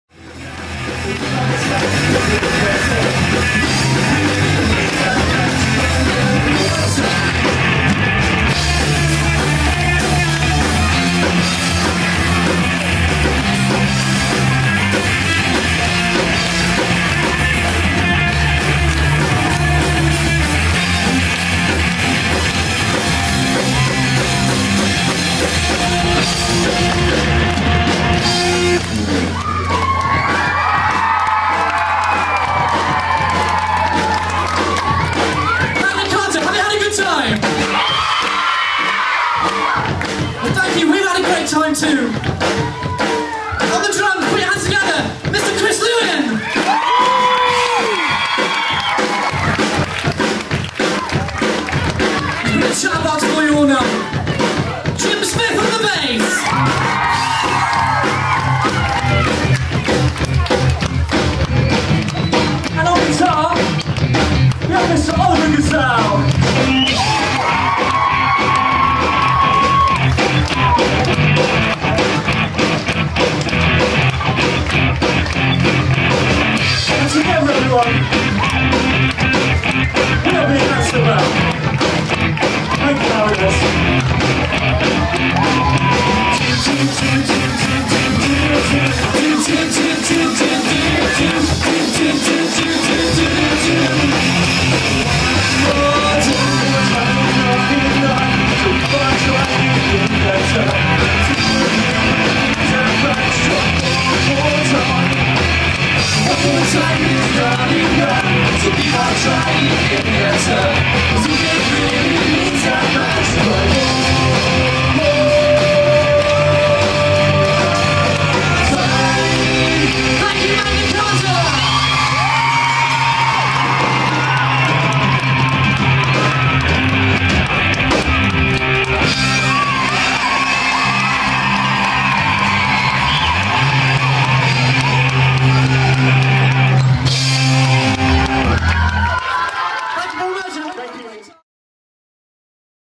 The Magna Carta School
*Sorry! This footage was filmed by a mobile phone so we apologise for the quality loss.*

Answer Back close their set at The Magna Carta School.

VIDEO CREDITS
Filmed by Ian Ginn
Thumbnail Picture by Chris Edwards
Special thanks to The Magna Carta School
Edited by Oliver Grizzell